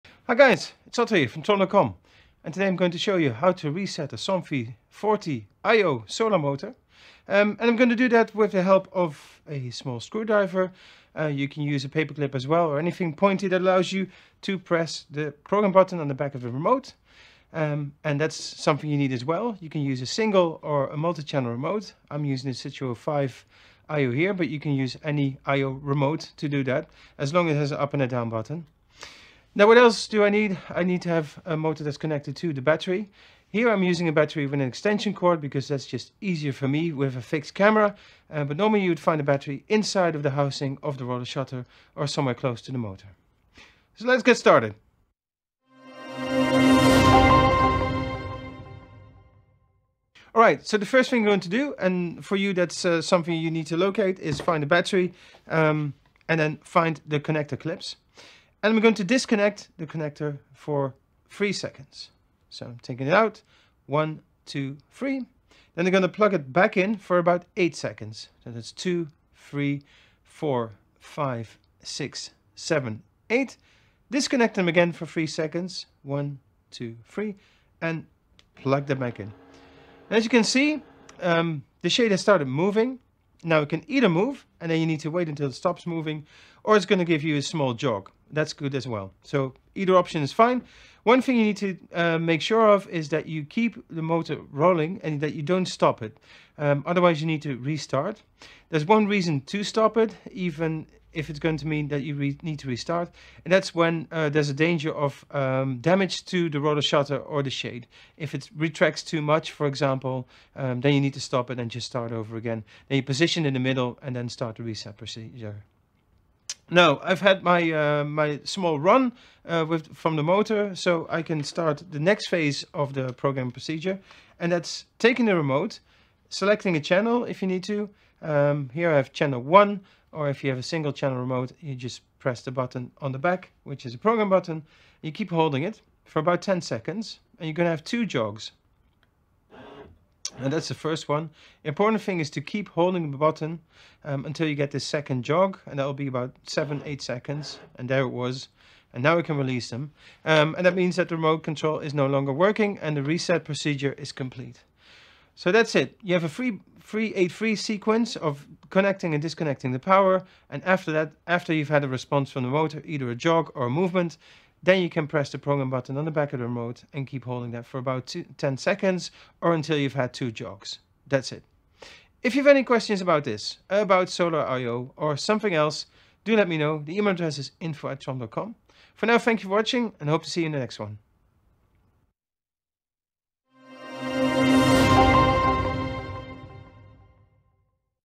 How to reset the Somfy Oximo 40 solar IO motor
In this video, we show you how to reset the Somfy IO solar motor, which is handy when things go wrong and you want to make a fresh start./